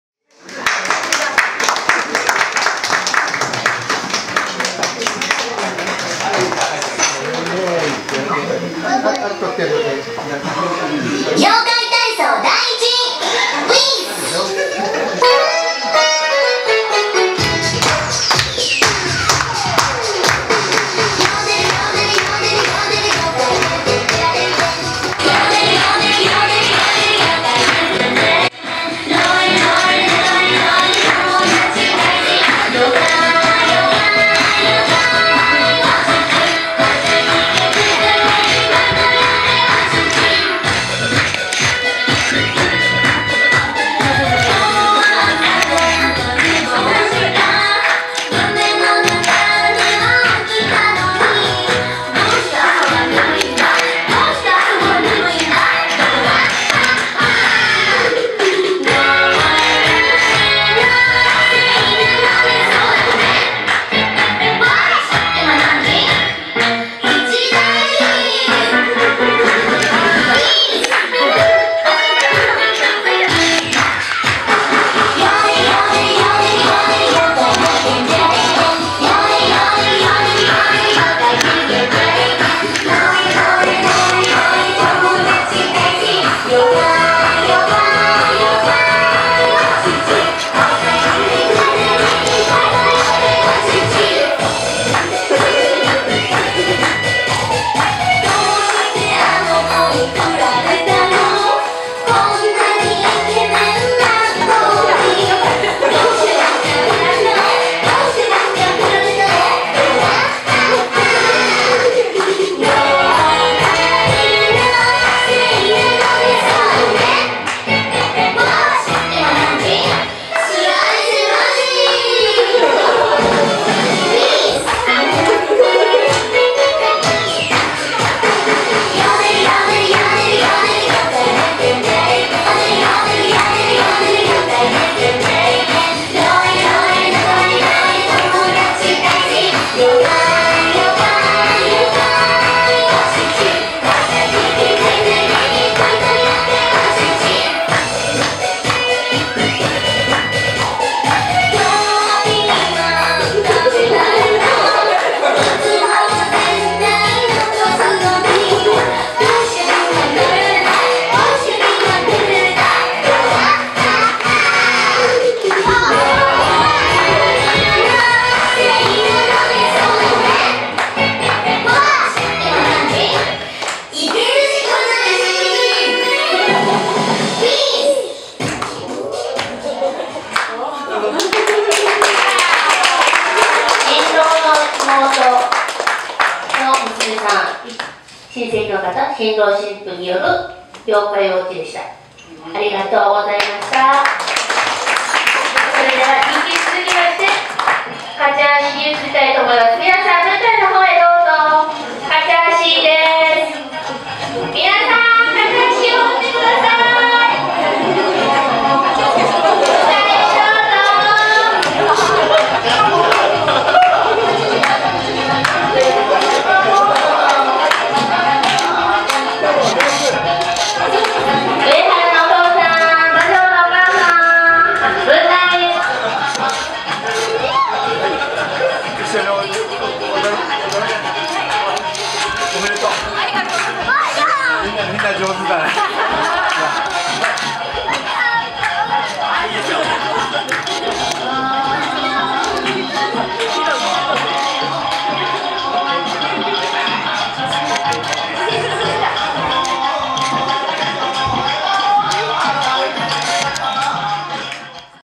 沖縄の結婚式の余興に突如新郎新婦が乱入【神宮会館のニービチ】
【那覇市波上宮まえ神宮會館のニービチ】
披露宴_N18312(n)